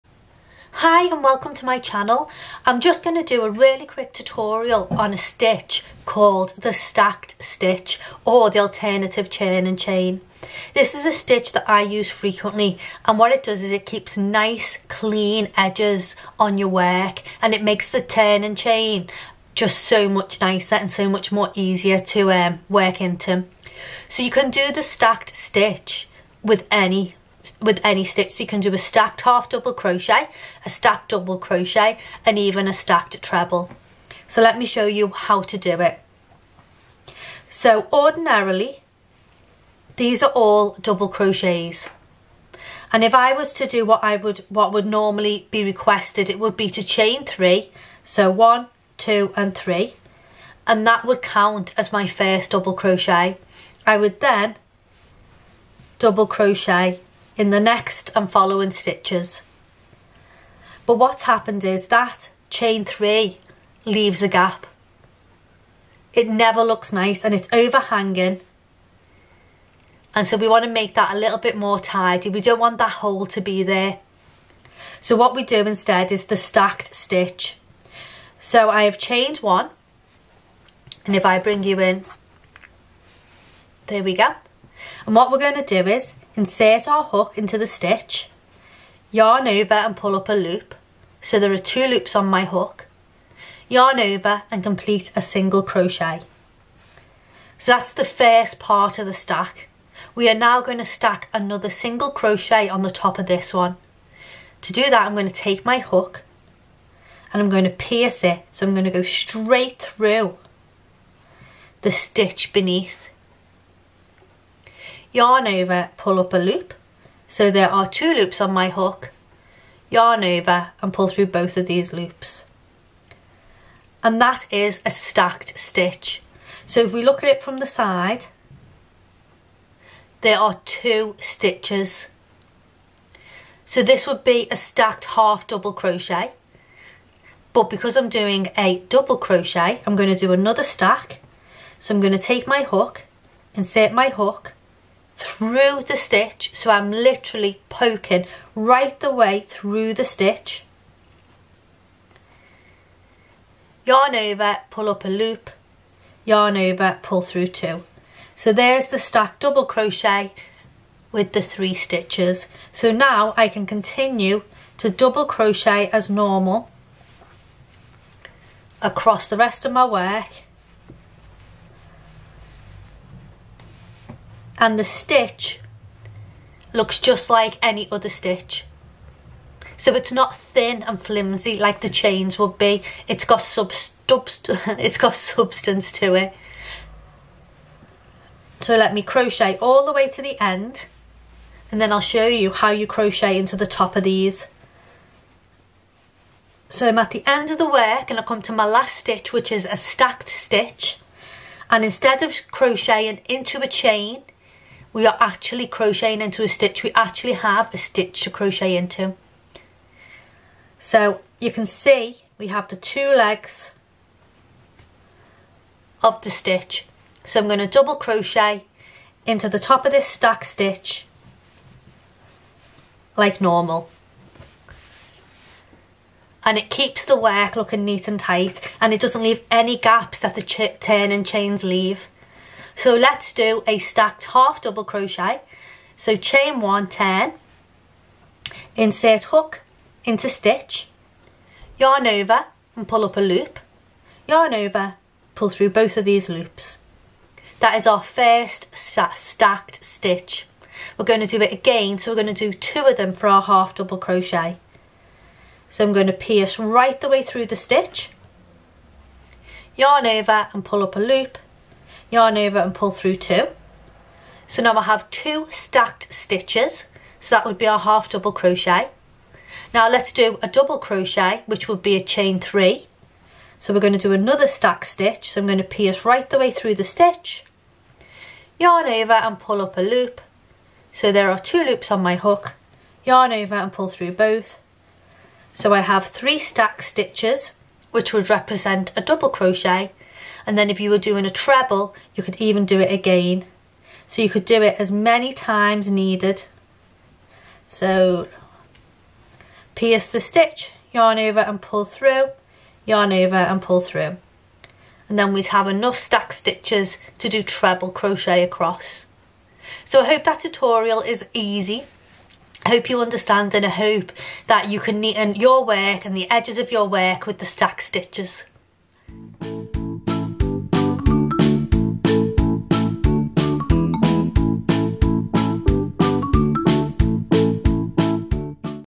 How To Do A Stacked Double Crochet | Alternative Turning Chain | Standing Double Crochet
Learn how to crochet a stacked stitch also known as an alternative turning chain of a standing stitch.

Sick of having uneven edges on your projects and turning chains leaving holes in your work? If so then this stitch is perfect for you. The stacked stitches are made instead of your regular turning chains and they leave straight edges and most importantly no holes.

The stacked stitch can be substituted for any crochet stich, and I think once you start using it you will never go back to regular turning chains again.

💛Etsy Pattern Store:

💛Etsy Custom Store:

Much Love,

Teenie Crochet x